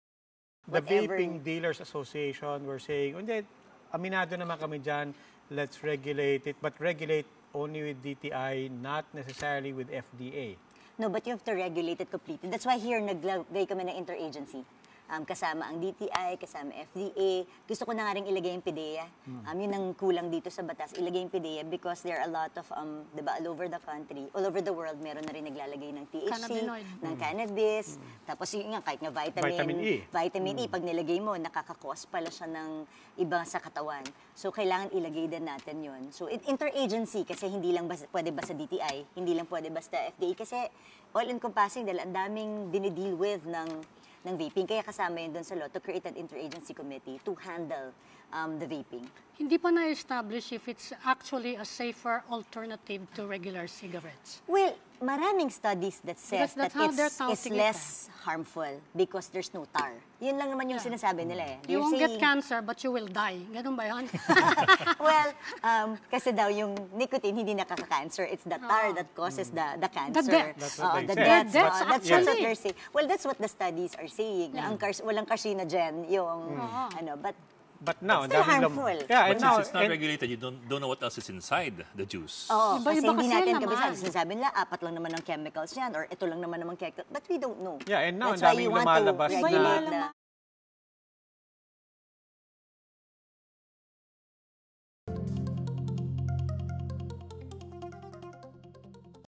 PDEA in regulating vaping products
"Gusto ko na rin nga rin ilagay ang PDEA."

Rep. BernadetteHerreraDy of the Bagong Henerasyon Party-list says she wants the Philippine Drug Enforcement Agency onboard to help regulate VapingProducts. She says this is due to some cases where other harmful or addictive substance are included in being sold to users.